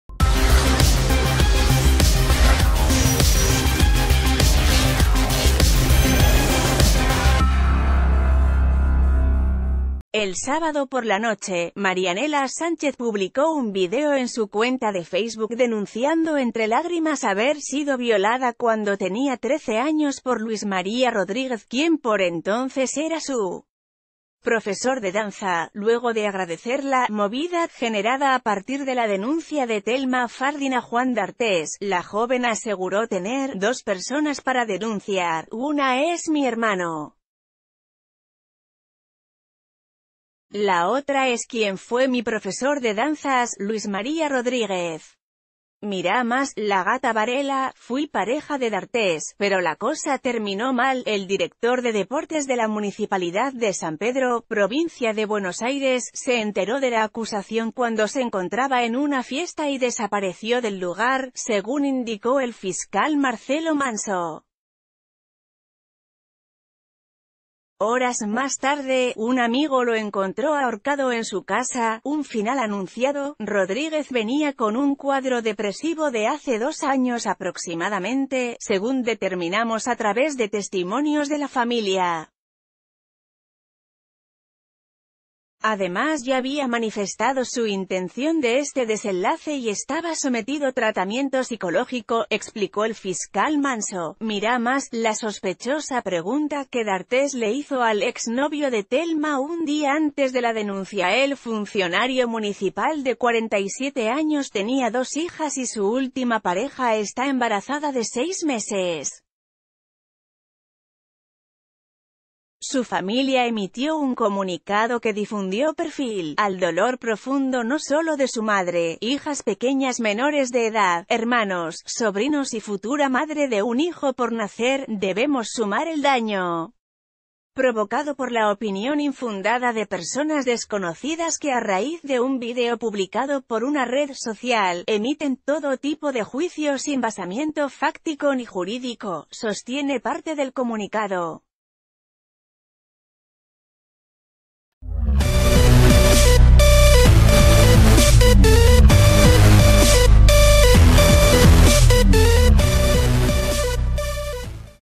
Encontraron ahorcado a un funcionario municipal denunciado por haber violado a una niña
El sábado por la noche, Marianela Sánchez publicó un video en su cuenta de Facebook denunciando -entre lágrimas- haber sido violada cuando tenía 13 años por Luis María Rodríguez, quien por entonces era su profesor de danza. Luego de agradecer la "movida" generada a partir de la denuncia de Thelma Fa...